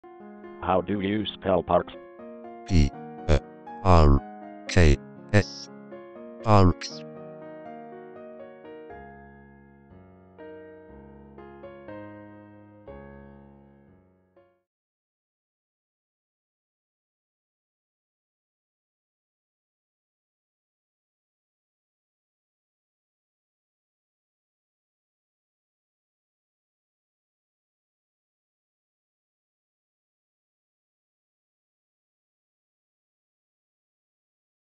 How do you spell parks?
Learn how to spell and pronounce parks.